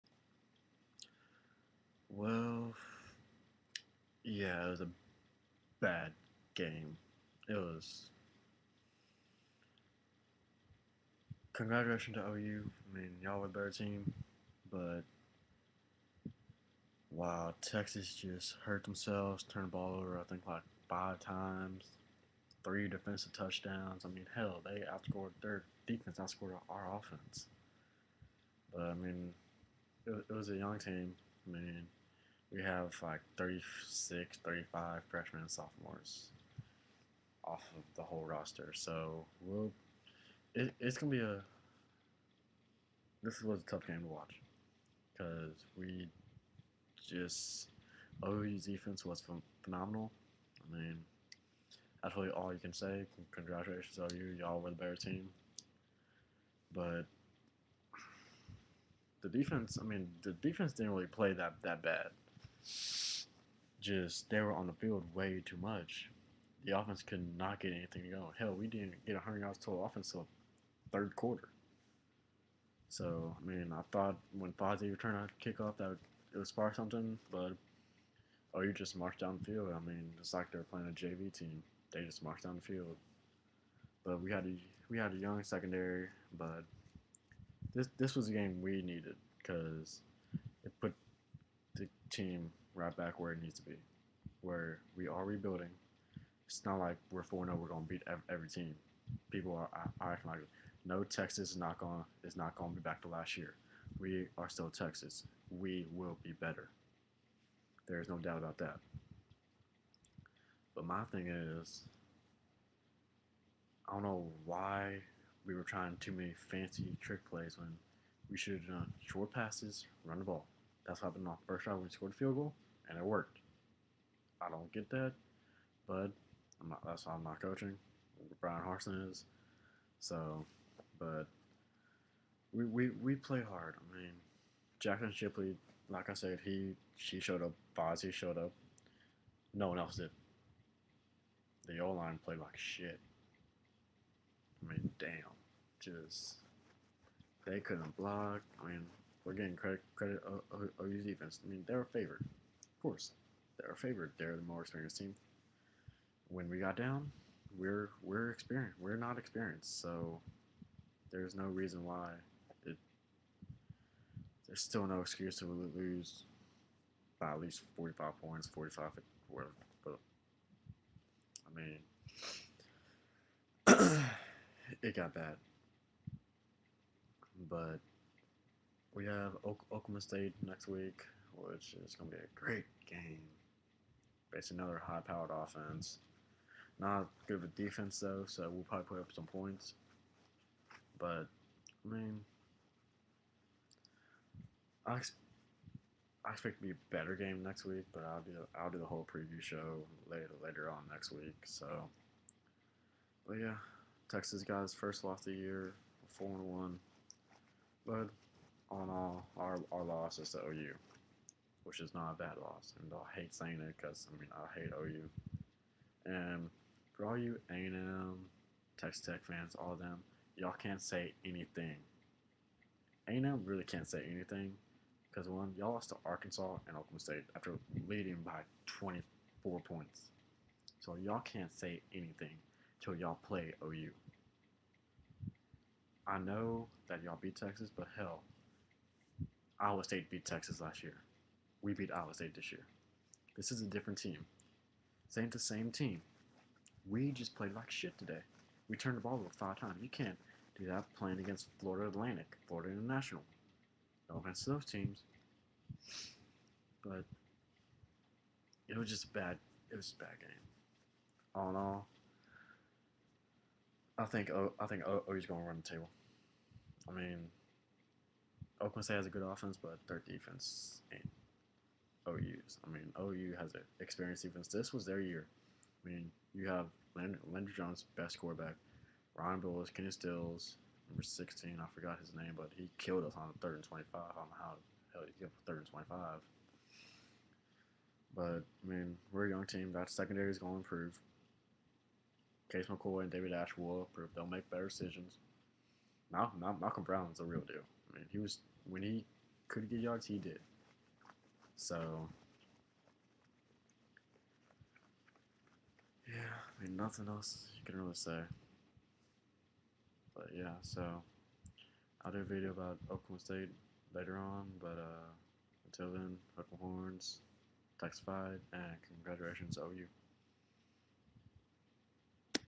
Texas Longhorns 17 oklahoma sooners 55
recap of texas longhorns loss to the sooner 17-5